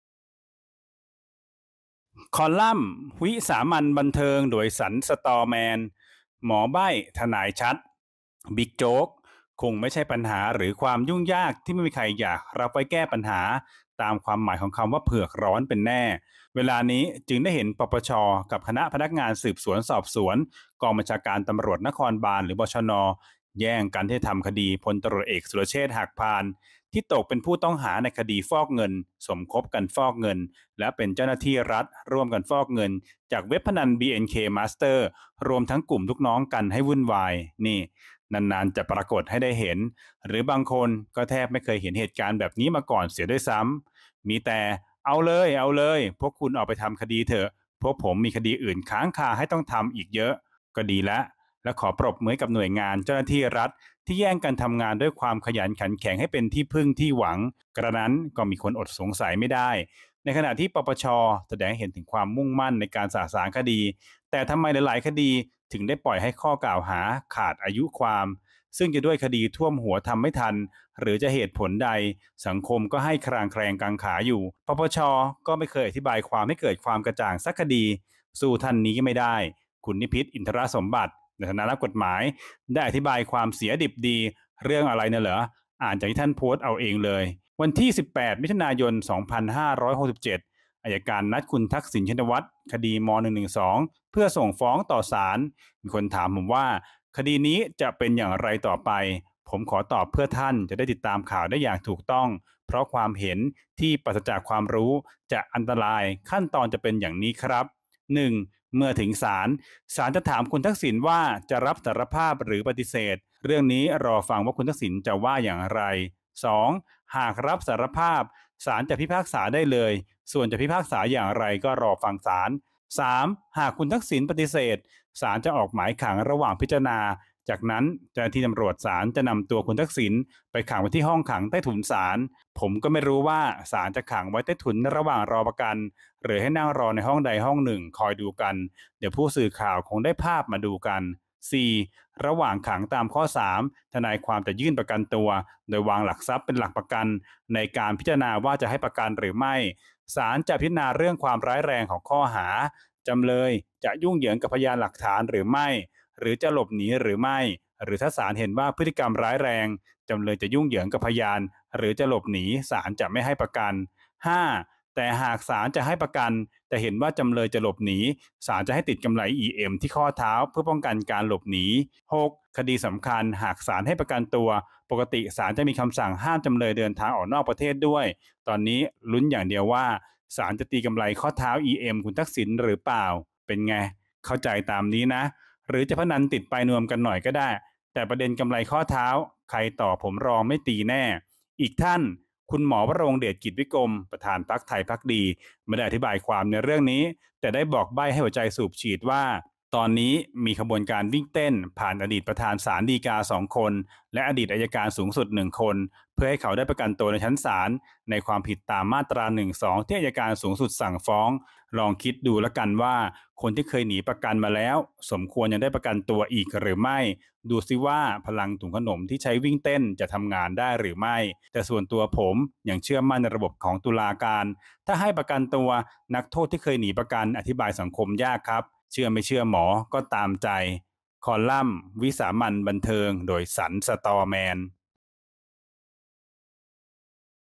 วิสามัญบันเทิง : หมอใบ้..ทนายชัด!
“บิ๊กโจ๊ก”..

คง..ไม่ใช่ปัญหาหรือความยุ่งยากที่ไม่มีใครอยากรับไว้แก้ปัญหา ตามความหมายของคำว่า “เผือกร้อน” เป็นแน่

เวลานี้จึงได้เห็น ป.ป.ช. กับคณะพนักงานสืบสวนสอบสวน กองบัญชาการตำรวจนครบาล (บช.น.) แย่งกันที่จะทำคดี พล.ต.อ.สุรเชษฐ์ หักพาล..

ที่ตกเป็นผู้ต้องหาในคดีฟอกเงิน สมคบกันฟอกเงิน และเป็นเจ้าหน้าที่รัฐร่วมกันฟอกเงิน จากเว็บพนัน BNK Master รวมทั้งกลุ่มลูกน้องกันให้วุ่นวาย!

นี่..นานๆ จะปรากฏให้ได้เห็น หรือบางคนก็แทบไม่เคยเห็นเหตุการณ์แบบนี้มาก่อนเสียด้วยซ้ำ..